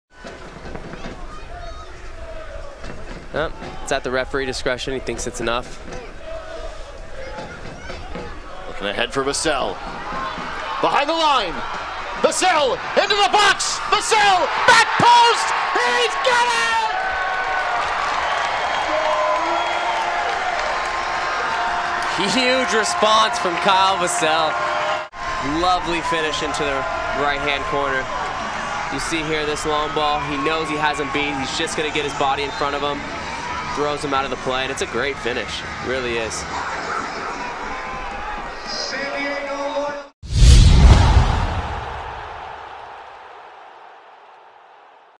Kyle Vassell with a Goal vs. Louisville City FC
Kyle Vassell (San Diego Loyal FC) with a Goal vs. Louisville City FC, 04/16/2022